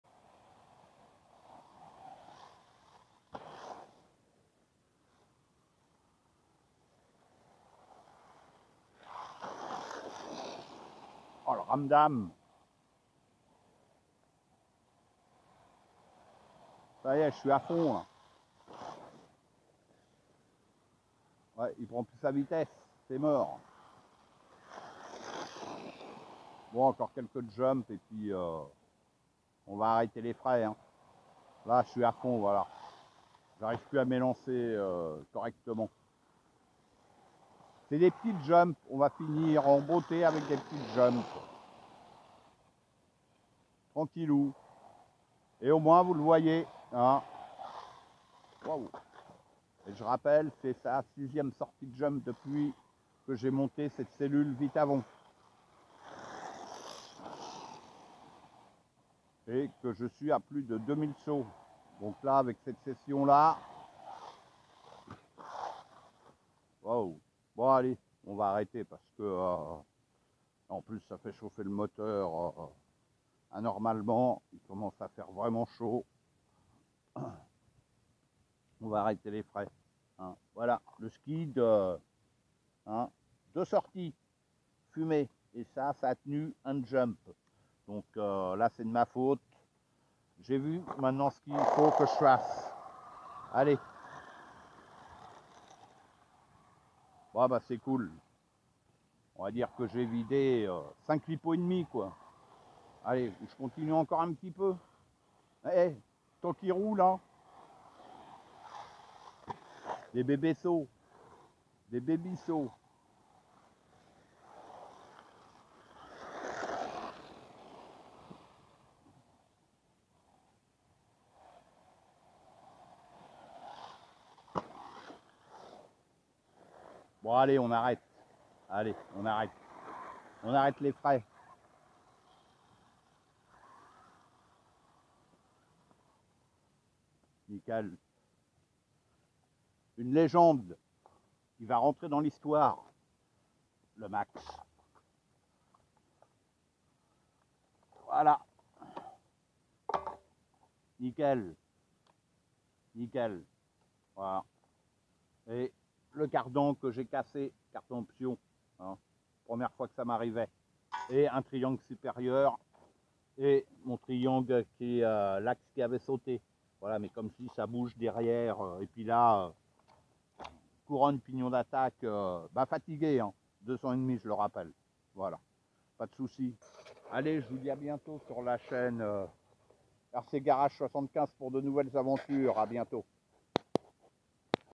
Maxx Vita & fin il en peu plus - Part 23
& 705 saut sur cette session du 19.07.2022 & au total depuis la pose de cette cellule sur 6 sortie 2768 saut & beaucoup beaucoup de violent choc, le Maxx v1 améliorer reste pour moi la meilleur rc jamais possédé et toutes mes vidéos live vous le montre.
La il va rentrer en atelier.